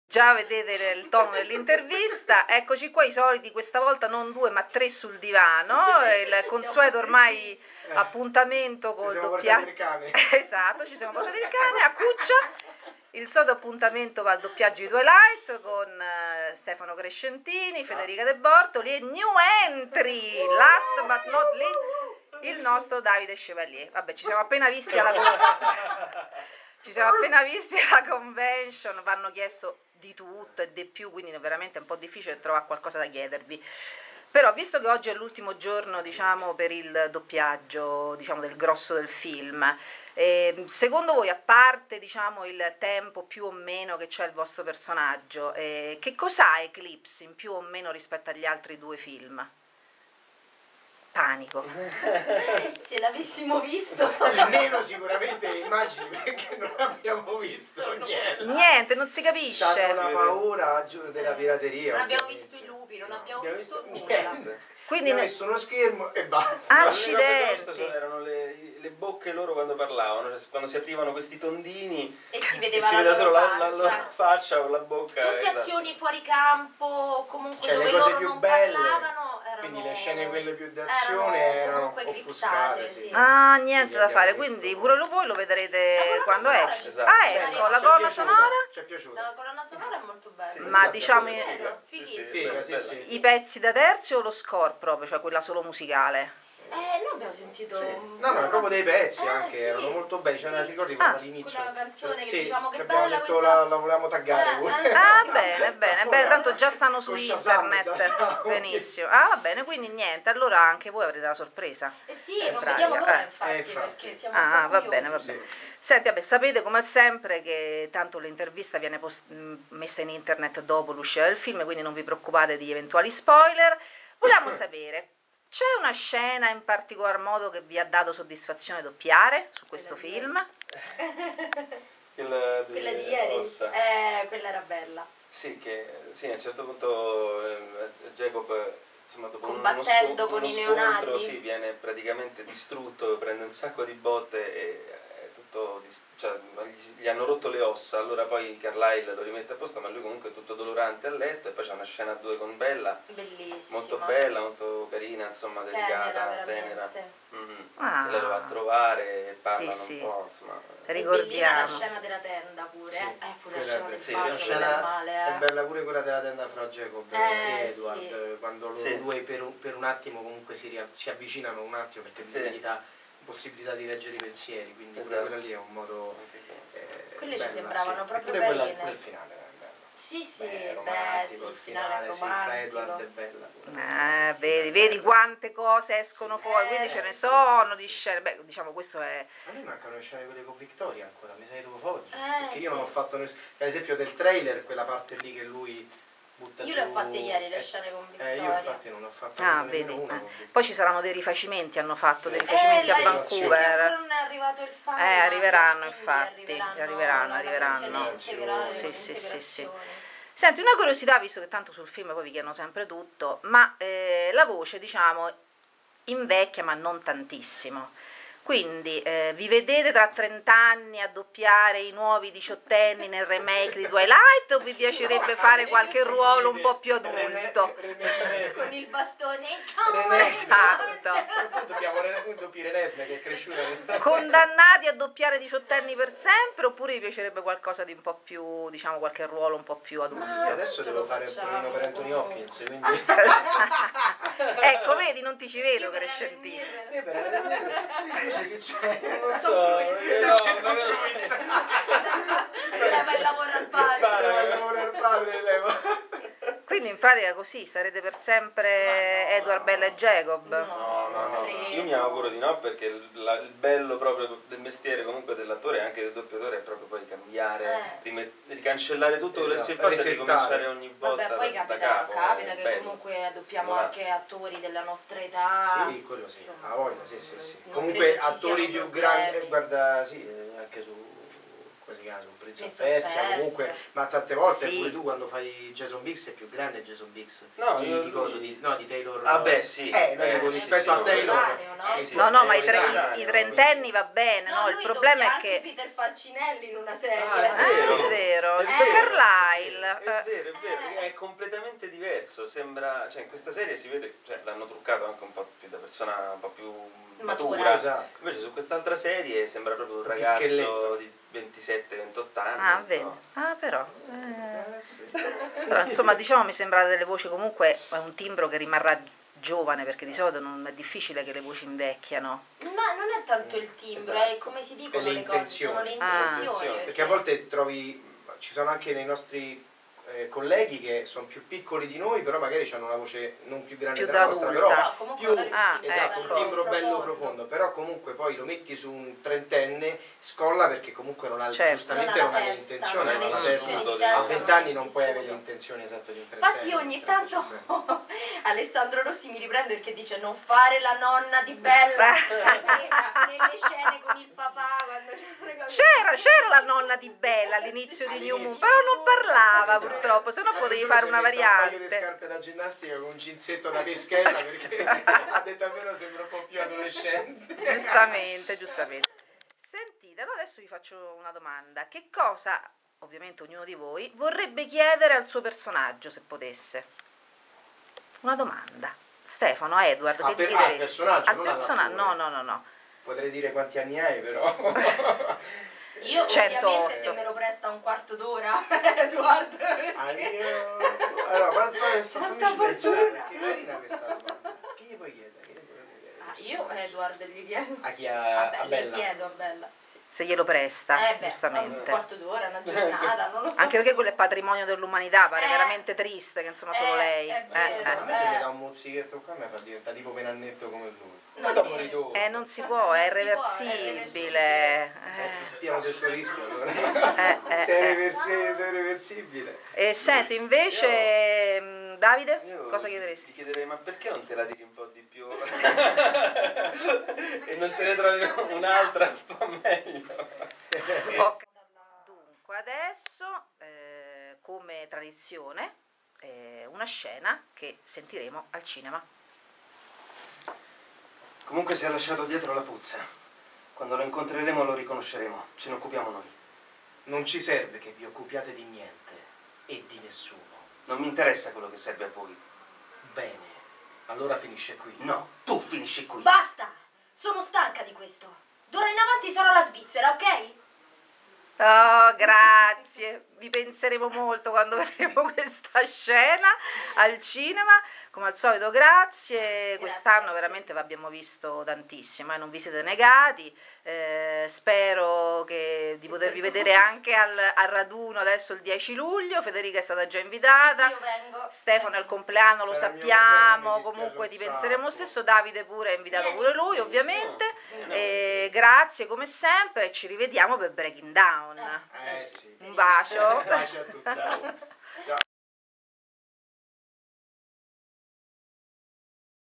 Eclipse incontro con i doppiatori
il consueto appuntamento al doppiaggio della twilight saga con Stefano Crescentini e Federica de Bortoli per il terzo capitolo..questa volta la coppia storica si e' arricchita della graditissima presenza di Davide Chevalier che presta la sua voce a Jacob.Grazie ragazzi, siete mitici!